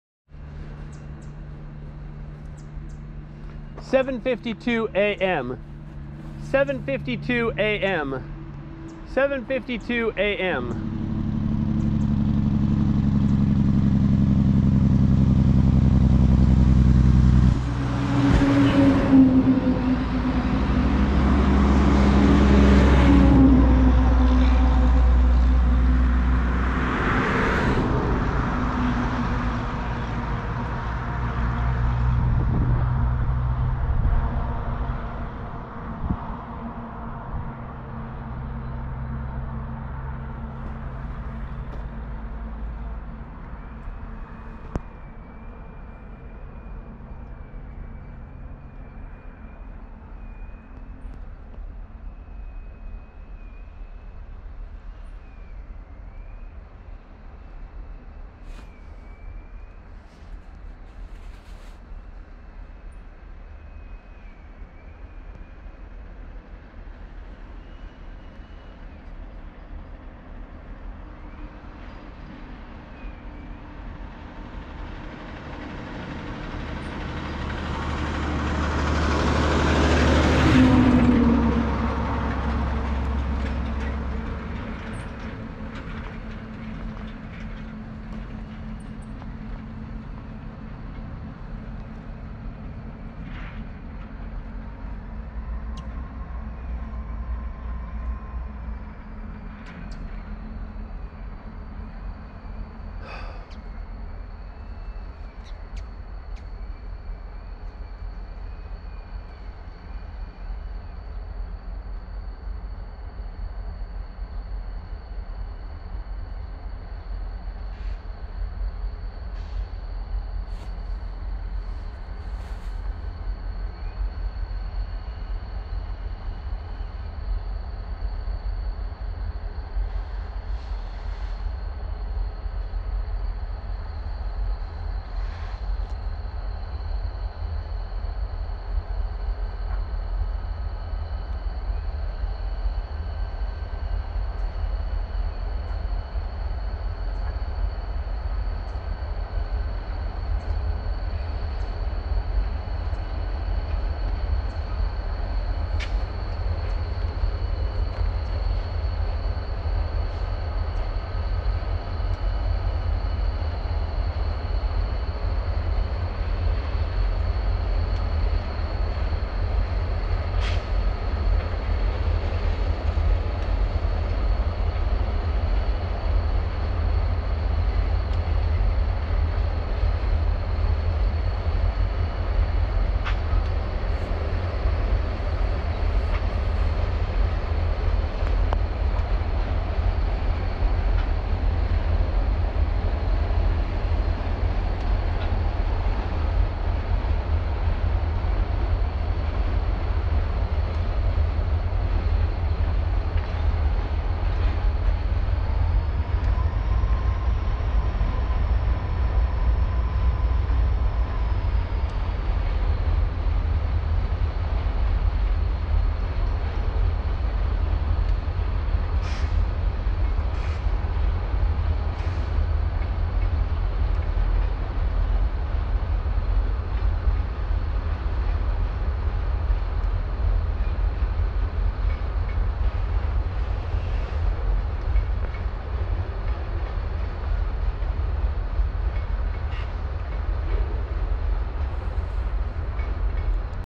Toledo, video 2, 5k 30fps, 2880p30
This is the train that runs from Toledo, Oregon to Albany.  Video 2 is at 7:52AM from Elk City Road at a cow pasture.